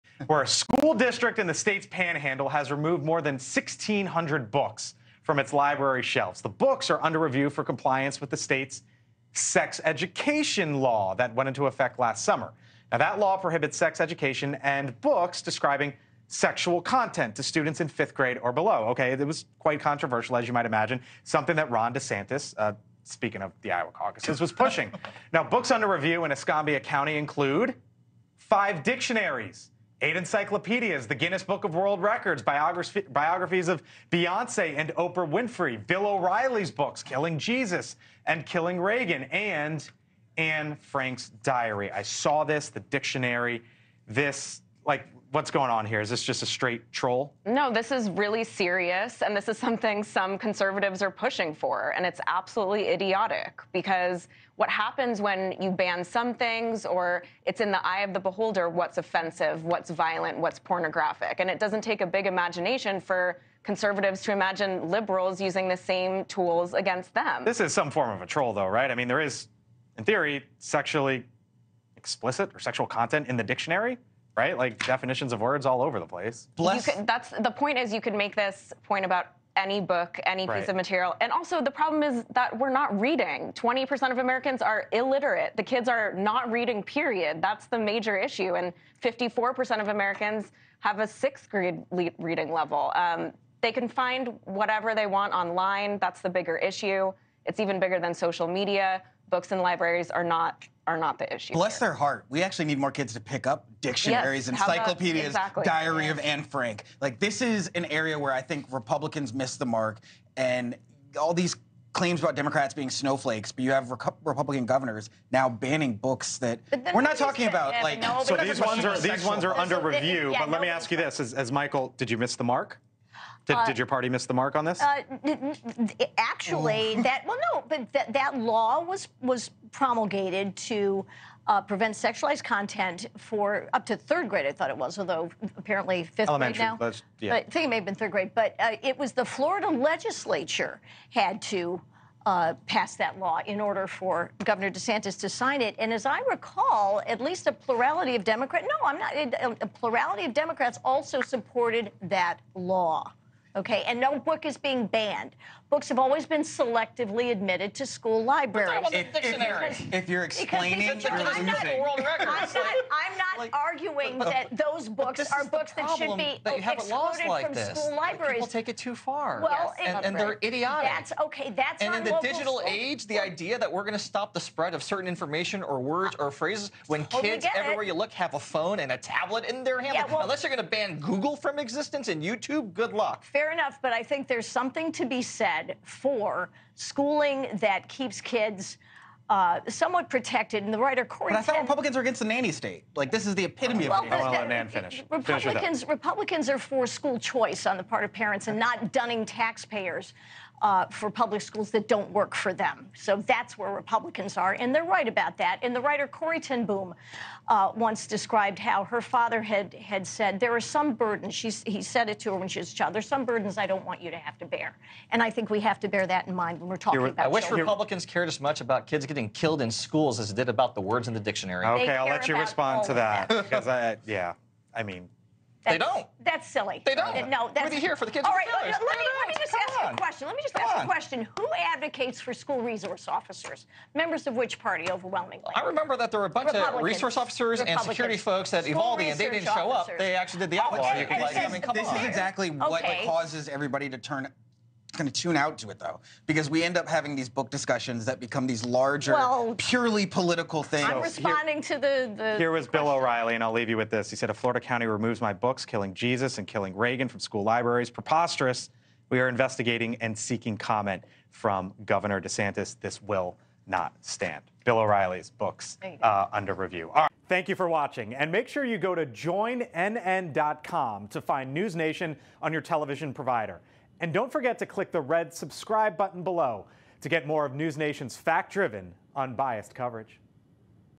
Dictionaries the latest target in Florida book ban | The Hill
A lawsuit can move forward against a Florida Panhandle school district over its removal of books about race and LGBTQ+ identities from library shelves, a federal judge ruled Wednesday.